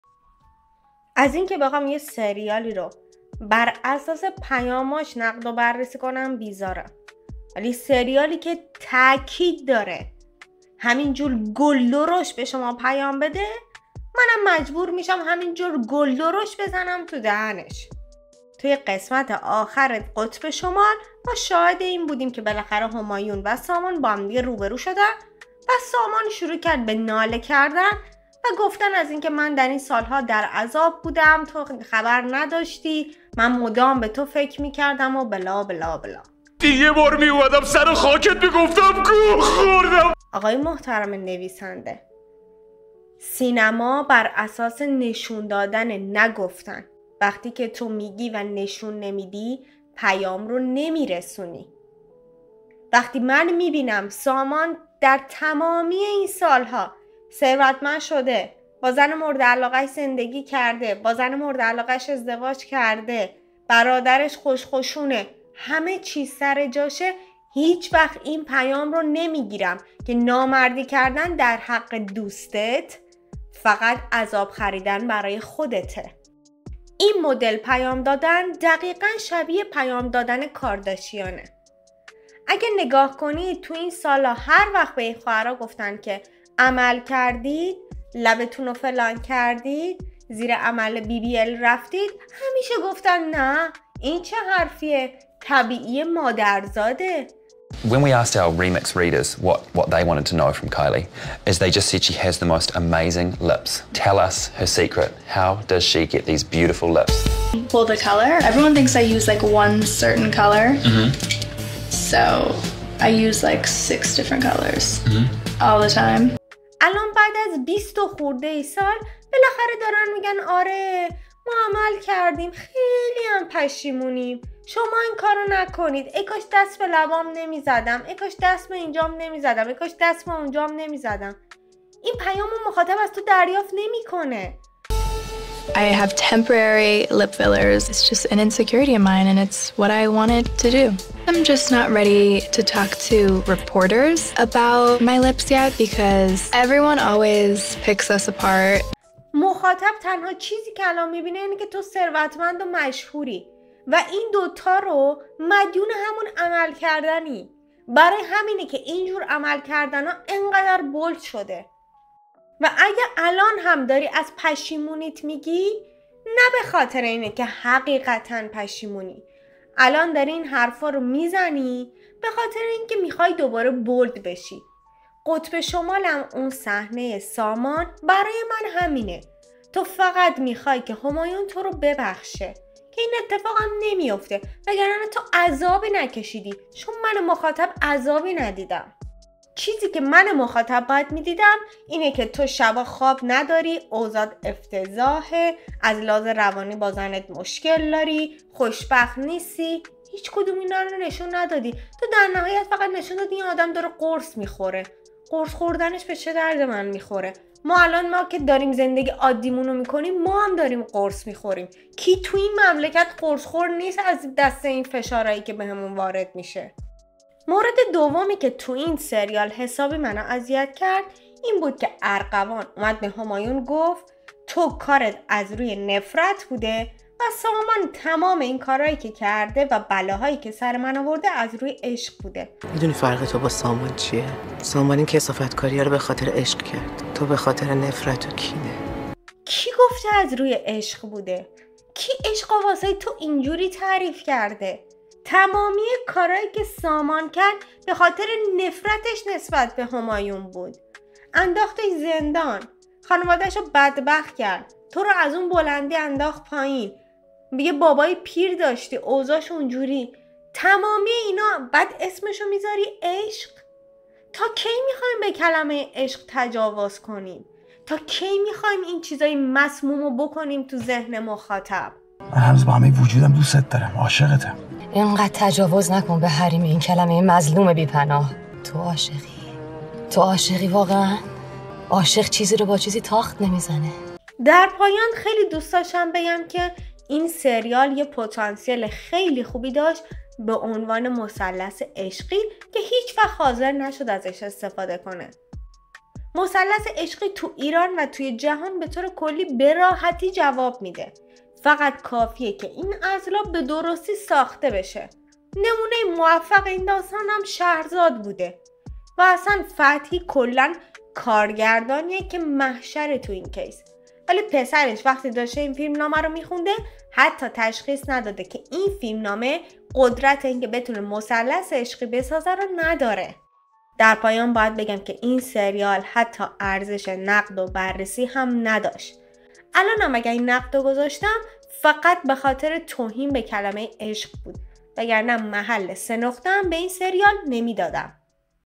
حضور کارداشیان ها در سریال قطب شمال! | بررسی قسمت آخر سریال قطب شمال
پیام گل درشت بود که از آسمون قطب شمال بارید! تو این عصر جدید فقط میشه گفت یکی یه همچین حرفایی میزنه و اونم خانواده کارداشیانه!
برای تولید این ویدیو از ویدیوهای زیر استفاده شده:
_  سریال شهرزاد
_ ویدیوی Top 10 Movie Love Triangles
_ ویدیوی KUWTK | Kylie Jenner Admits to Doing What to Her Lips?! | E!
با یک دسته پاپ کورن در دست، این سفر را همراه با من تجربه کنید.
🔰 ژانر: درام | عاشقانه
📆 سال تولید: 1402
📆 سال انتشار: 1403
👥 بازیگران: فرزاد فرزین، آناهیتا درگاهی، امیرحسین فتحی، نازنین بیاتی، فرهاد آئیش، فرزاد حسنی، عباس جمشیدی‌فر، شهرام قائدی، نگار فروزنده، شهروز دل‌افکار، روح الله زمانی، امیر دژاکام، بهادر زمانی و...
👤 کارگردان: امین محمودی
📄 خلاصه داستان: همایون و ارغوان در آستانه ازدواج، قربانی خیانتی هولناک می‌شوند. همایون پشت دیوارهای زندان برای انتقام از سامان لحظه شماری می‌کند که مرگ از راه می‌رسد. آرشام یکتا متولد می‌شود. دختری به نام صحرا کنارش قرار می‌گیرد تا مسیر انتقام از خائنان برای آرشام هموار شود...